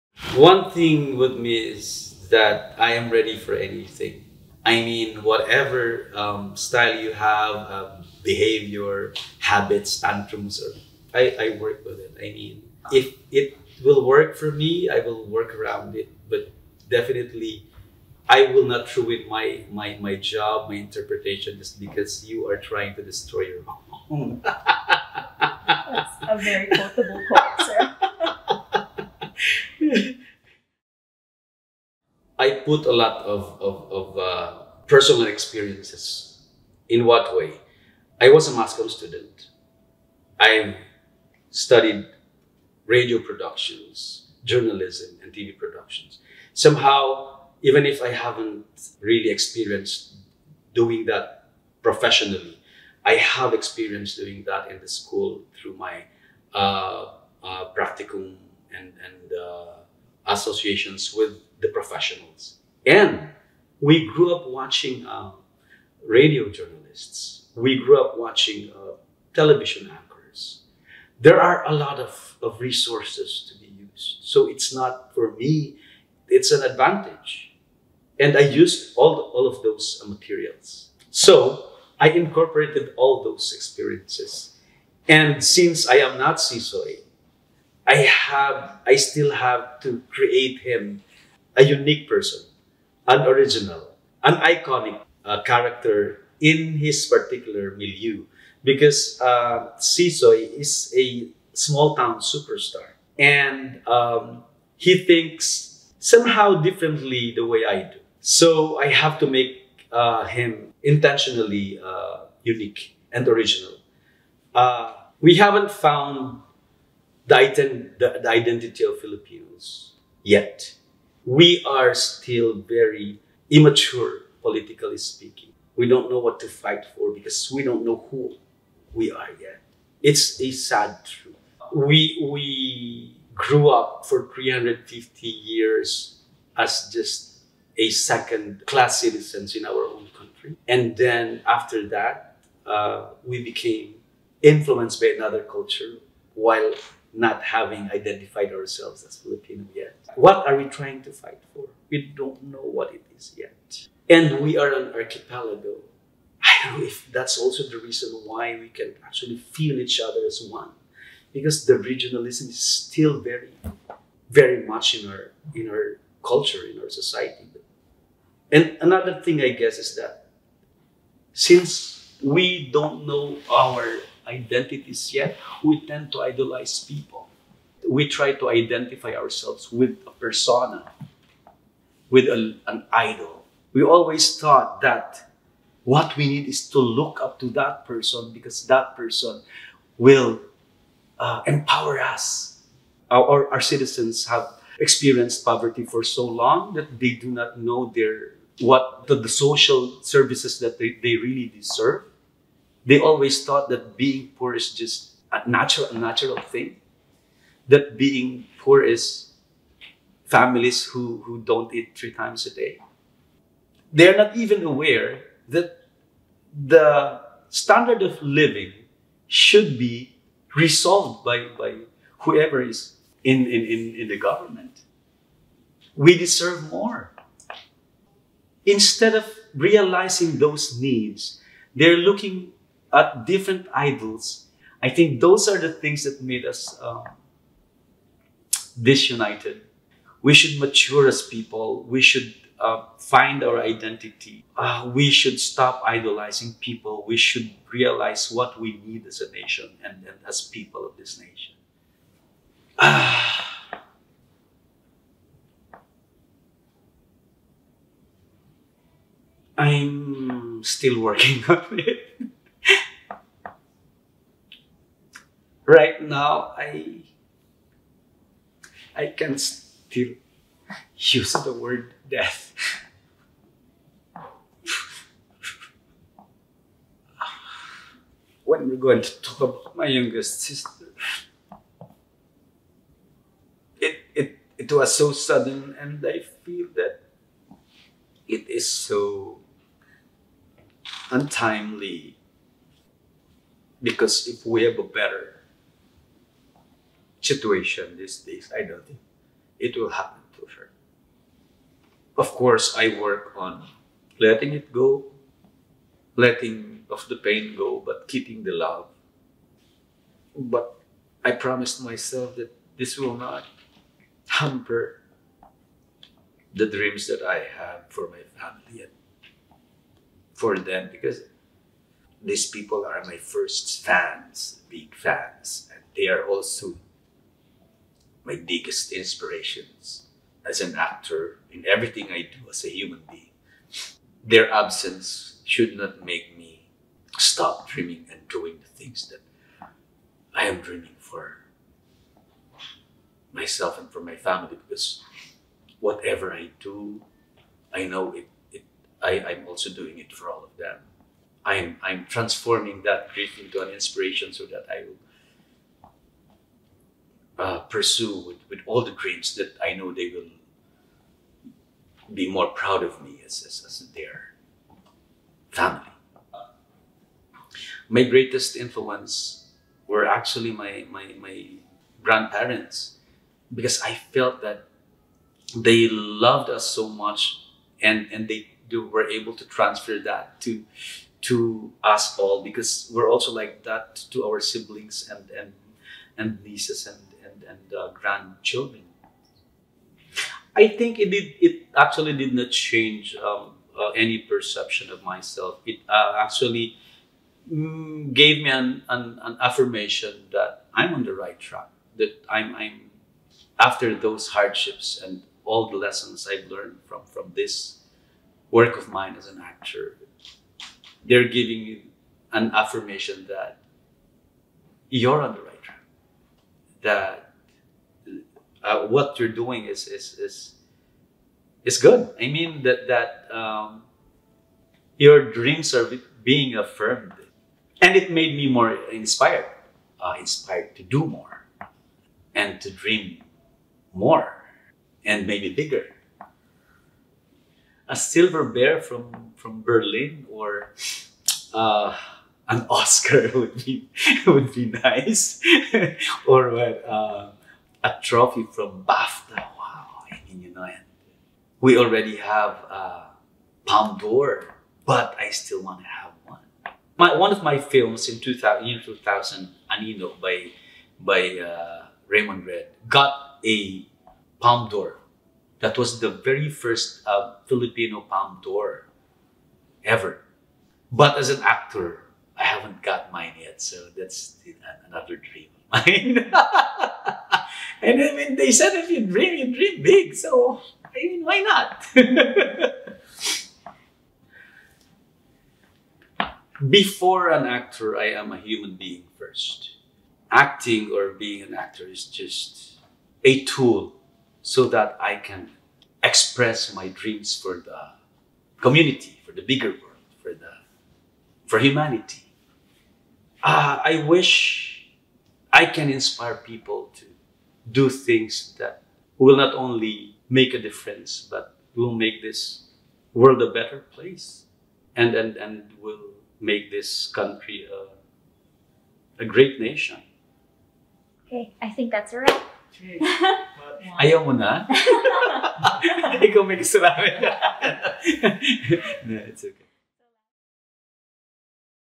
John Arcilla On Acting, Longevity, And Nation-Building | What I've Learned | Esquire Philippines
“I think Filipinos tend to idolize people, we tend to identify ourselves with a persona, instead of looking at ourselves as a group of people to determine our needs.”

Watch what John Arcilla, the Philippines' first-ever Volpi Cup awardee, had to say about what the country needs, his dreams of winning an Oscar and a Palme d’Or, and more.